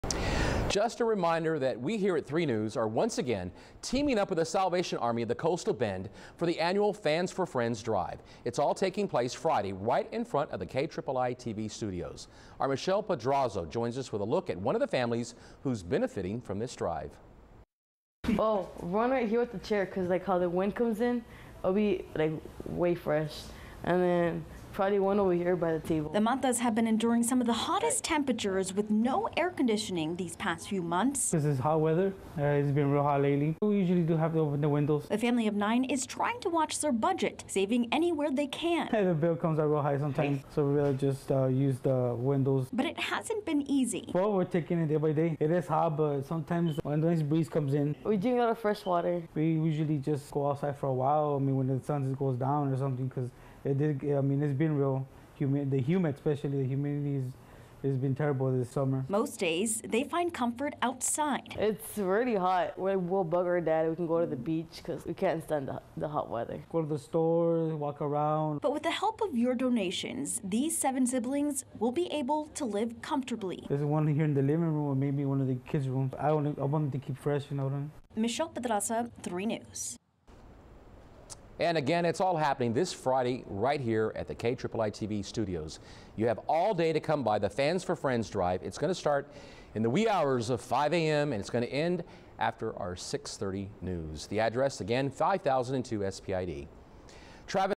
Fans for Friends Drive aims to help families in need stay cool this summer
Kiii-TV is once again teaming up with the Salvation Army of the Coastal Bend for the annual Fans for Friends Drive.